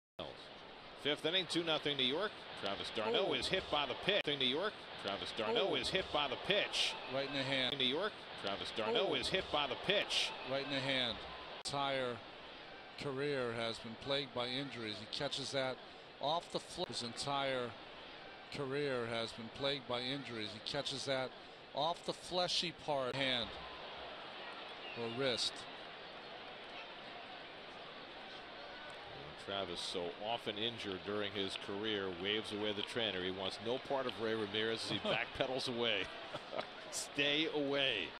Travis d'Arnaud gets hit on the hand with a pitch
After getting hit on the hand with a pitch, Travis d'Arnaud runs away from the Mets' trainer as he jogs onto the field to check on him